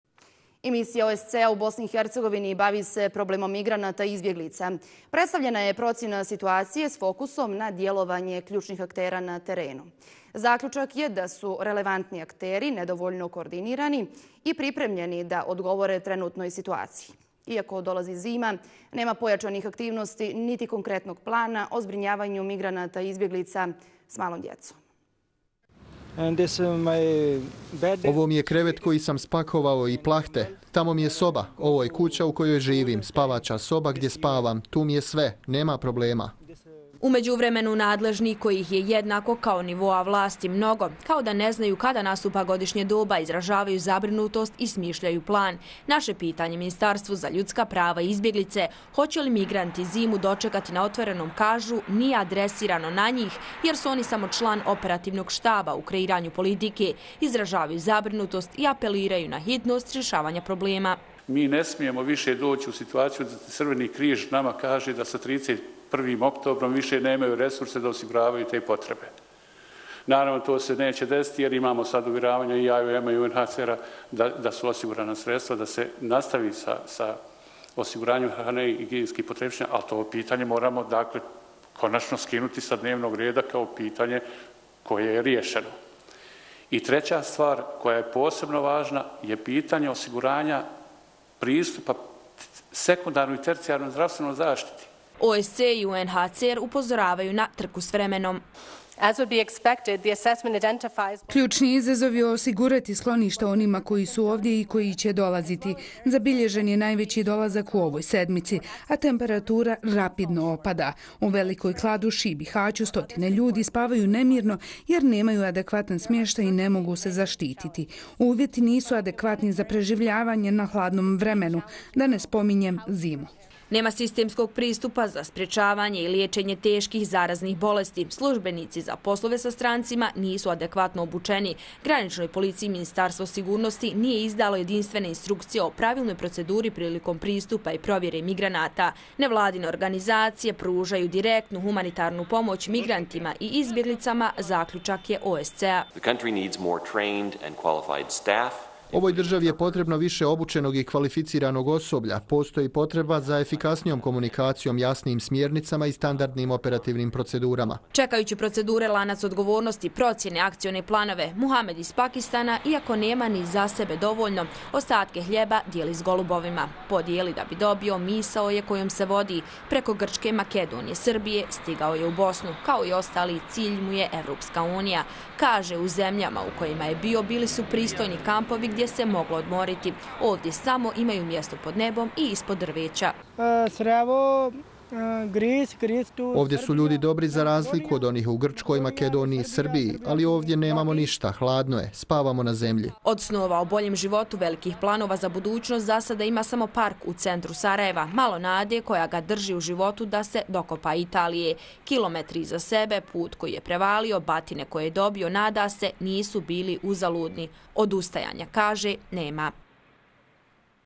OSCE: RELEVANTNI AKTERI U BIH NEPRIPREMLJENI ZA MIGRANTE (25 09 2018)
I Misija OSCE -a u BiH bavi se problemom migranata i izbjeglica. Predstavljena je procjena situacije s fokusom na djelovanje ključnih aktera na terenu. Zaključak je da su relevantni akteri nedovoljno koordinirani i pripremljeni da odgovore trenutnoj situaciji. Iako dolazi zima, nema pojačanih aktivnosti niti konkretnog plana o zbrinjavanju migranata i izbjeglica s malom djecom.